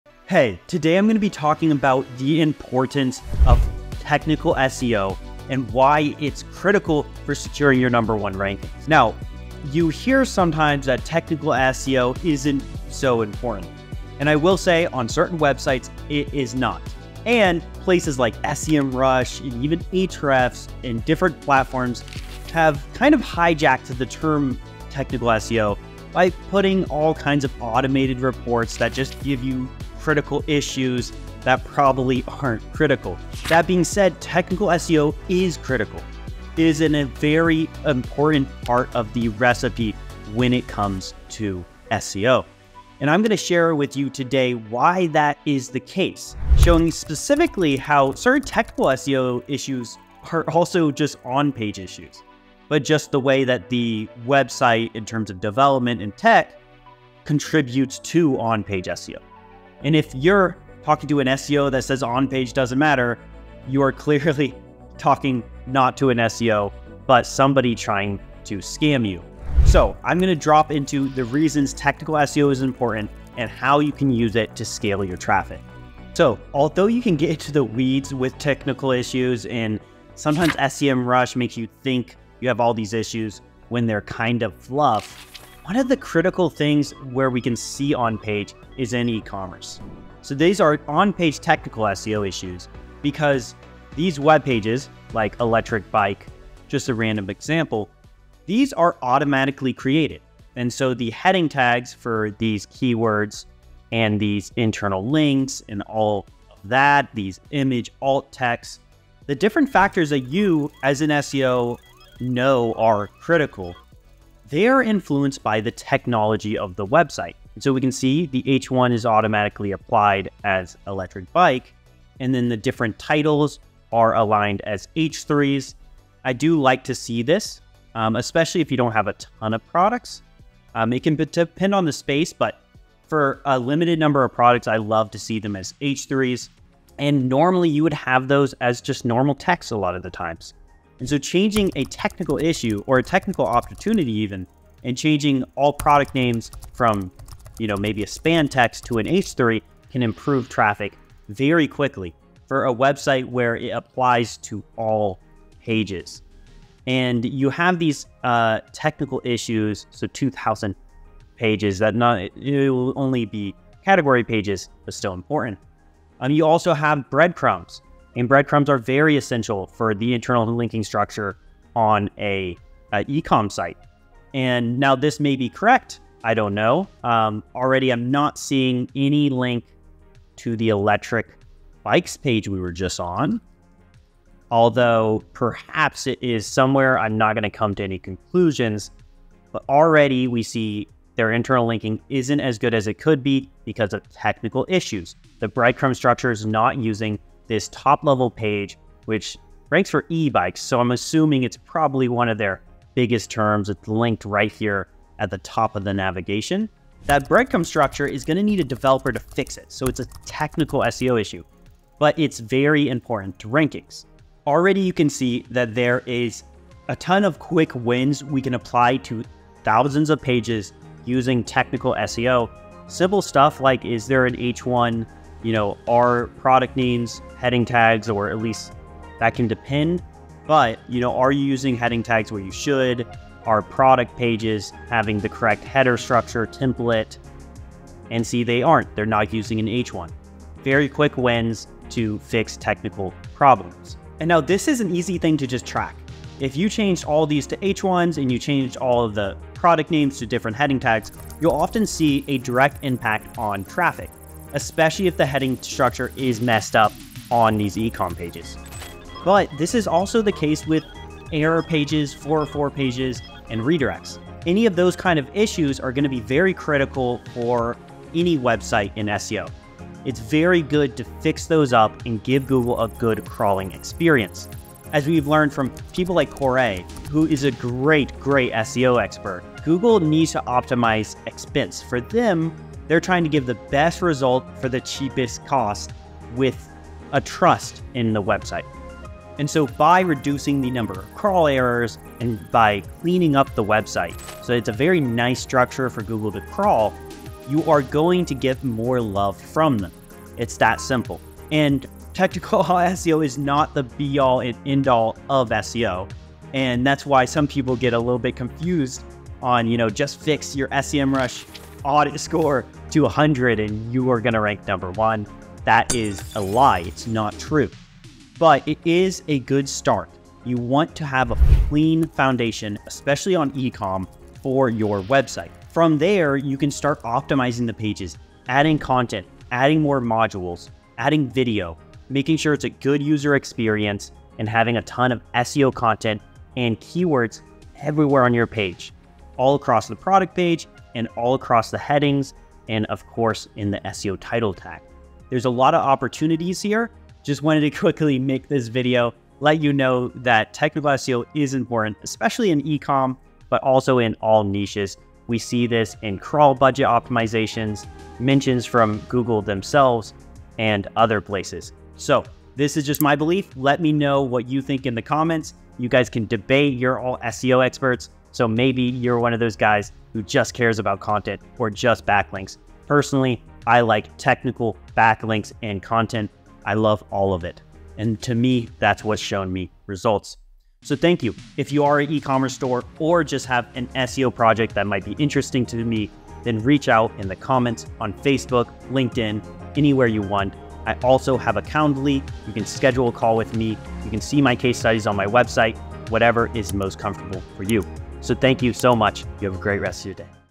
The Power of Technical SEO for E-Commerce Revenue 📈
Do you think of revenue when you hear technical SEO?

Not likely, but for e-Commerce, technical SEO can directly affect on-page SEO and your rankings on Google.

What Is Technical SEO?

Technical SEO is creating a clean foundation for your SEO that allows your website traffic to skyrocket looking at HTTP status codes and the technology behind your website.